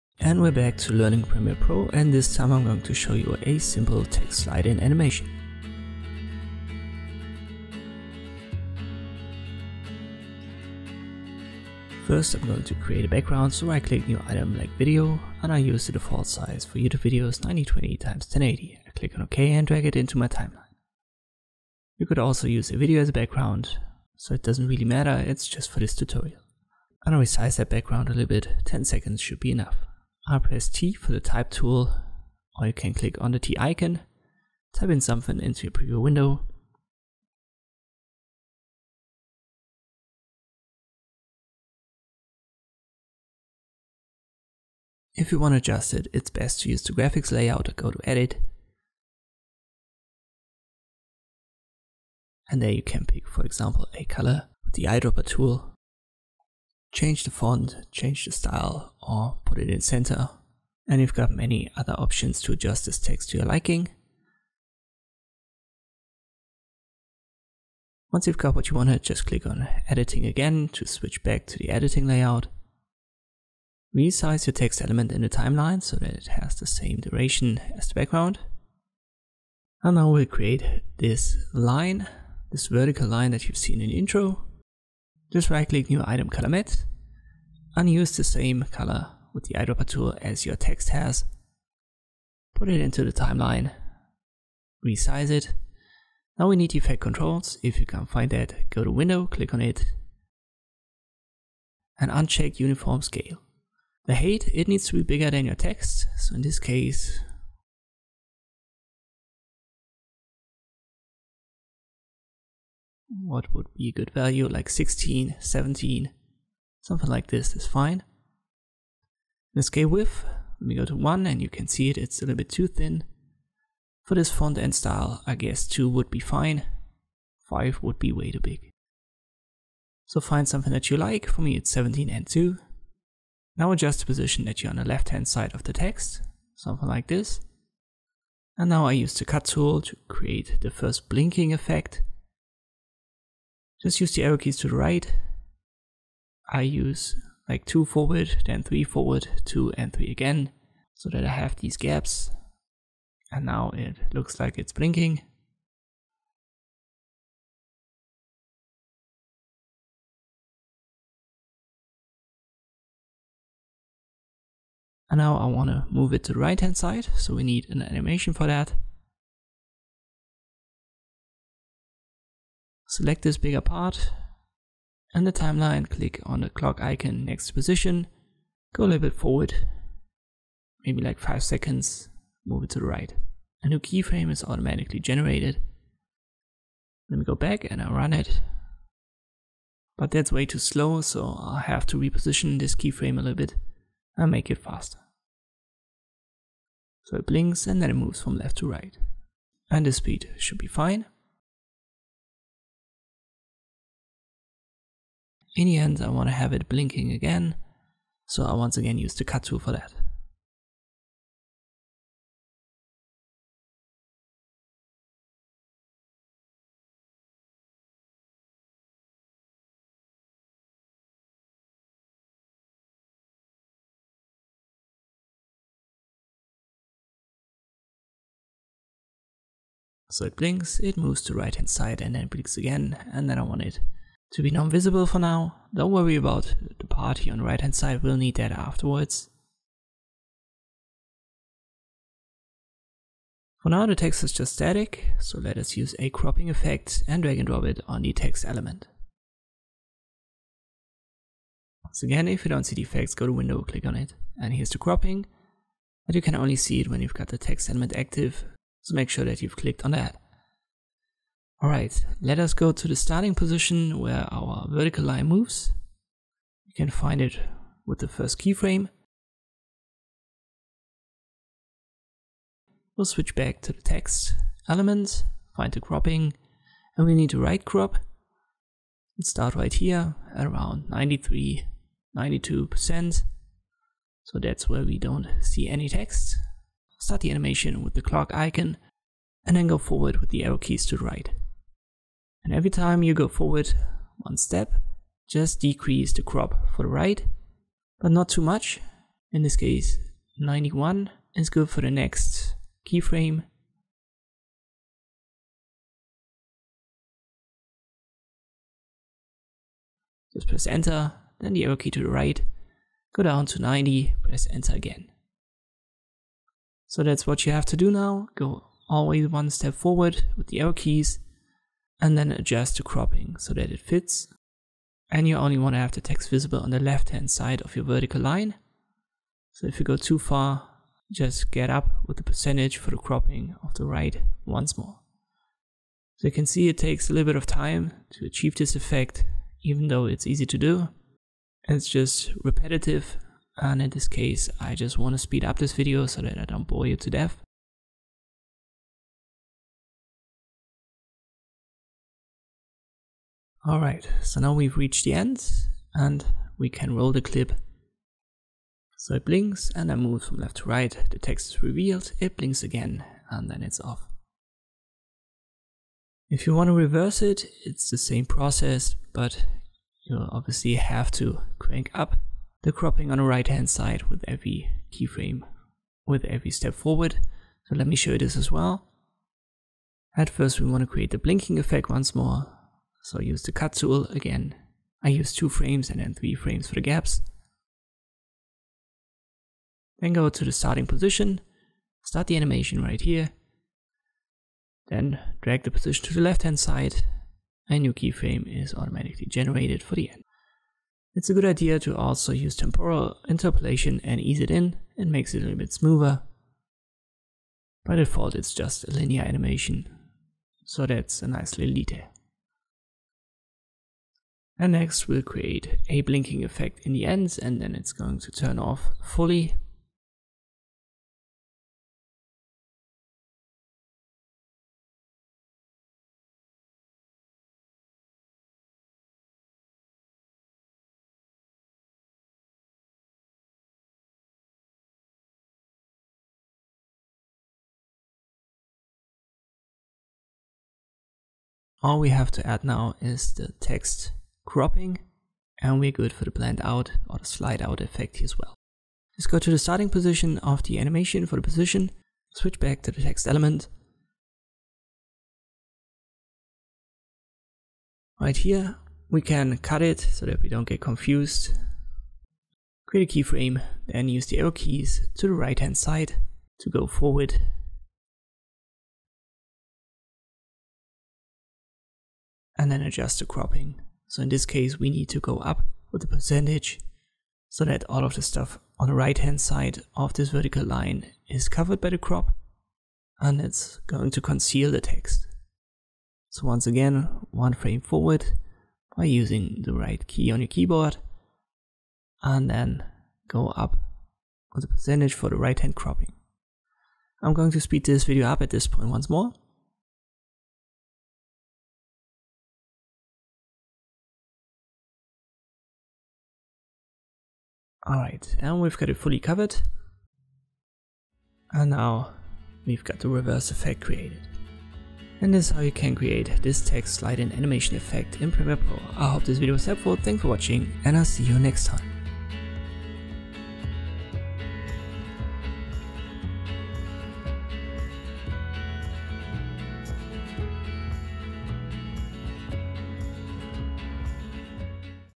Create a Text Slide In Animation in Premiere Pro
In this tutorial I'll show you how to create a text slide-in animation in Adobe Premiere Pro.